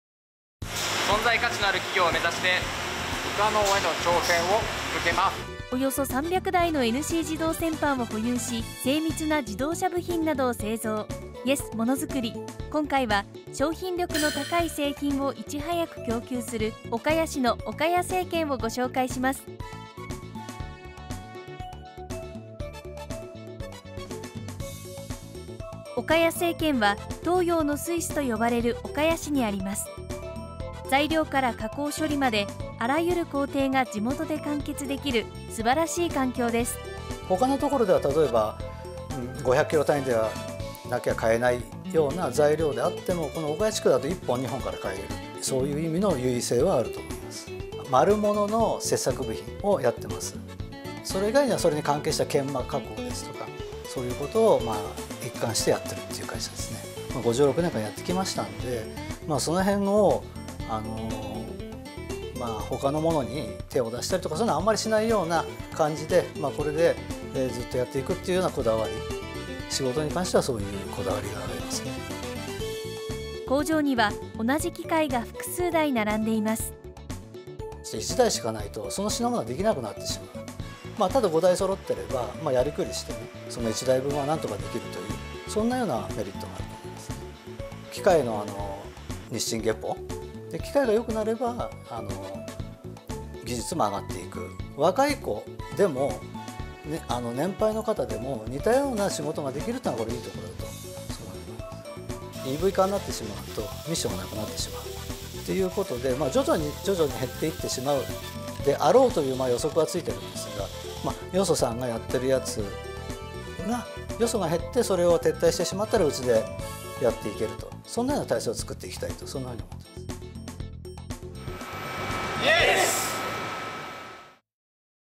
YES！ものづくり～岡谷セイケン①～長野県のものづくり企業に密着！
岡谷セイケンは、精密な自動車部品などを製造しており、東洋のスイスと呼ばれる岡谷市にあります。